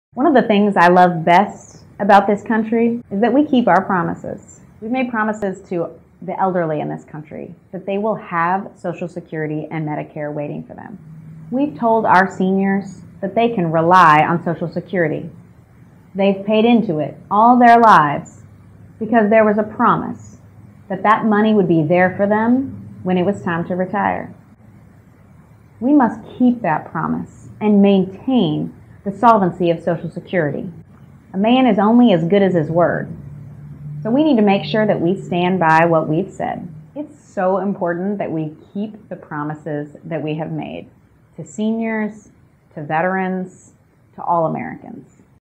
Social Security -- 'We must keep our promise'
Tabitha Isner talks about the importance of KeepingOurPromises, particularly to seniors. We must protect Social Security and Medicare.